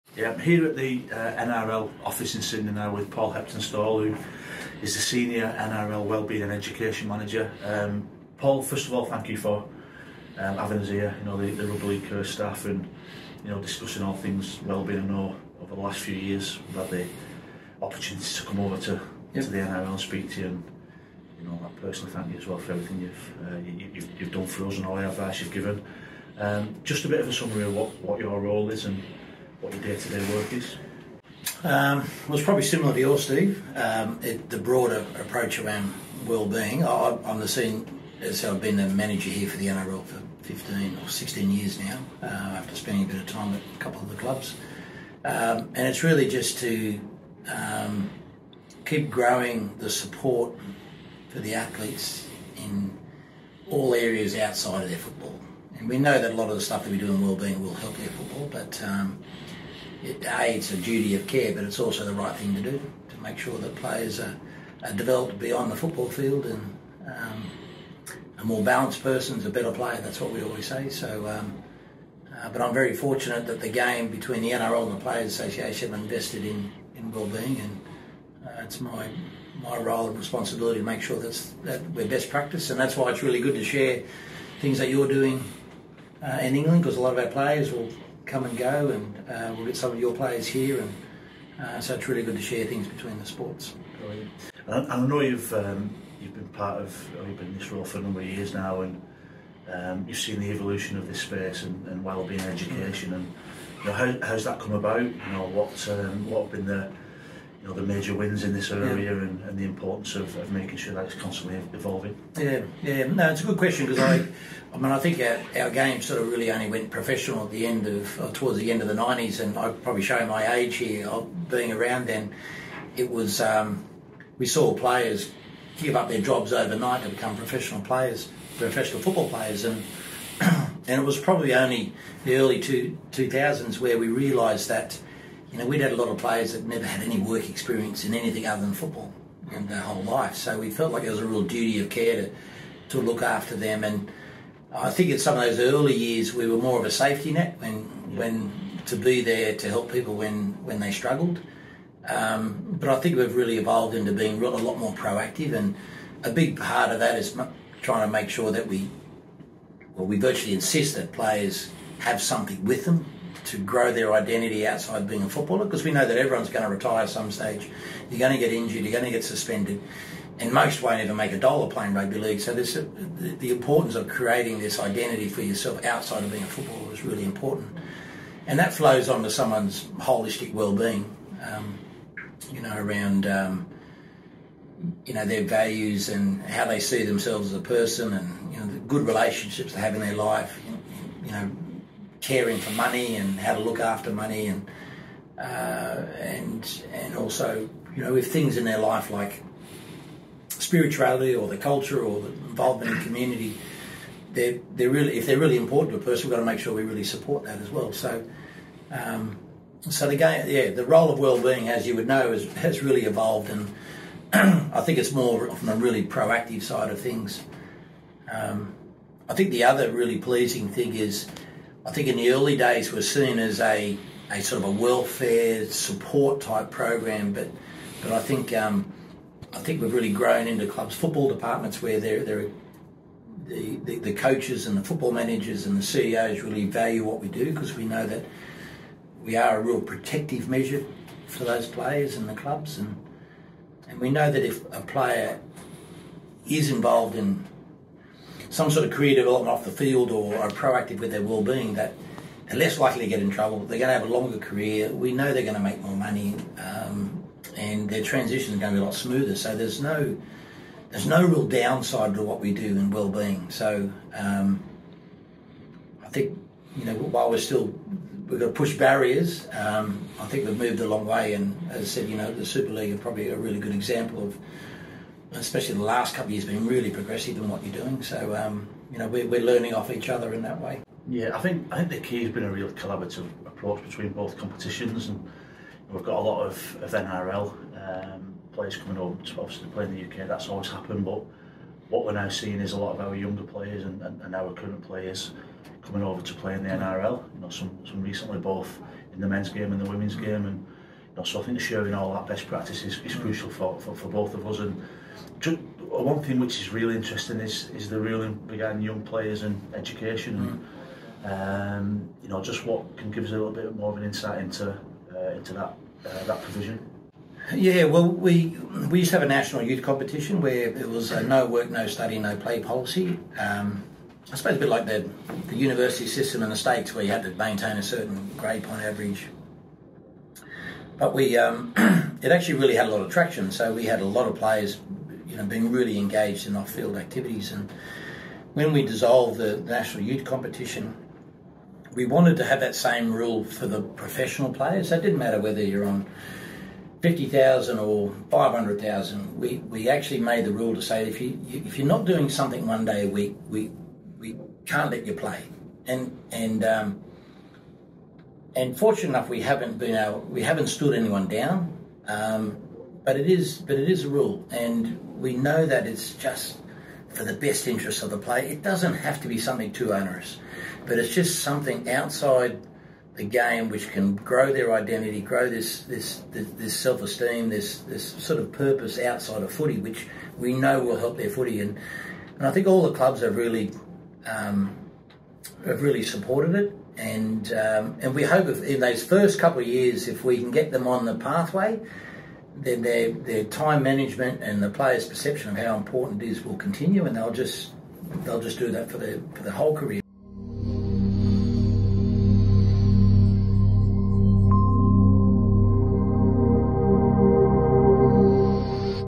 Steve McCormack and Paul Heptonstall
RL Cares Director of Welfare Steve McCormack and his counterpart in the NRL, Paul Heptonstall, chat about key issues around player welfare during Steve's recent trip Down Under.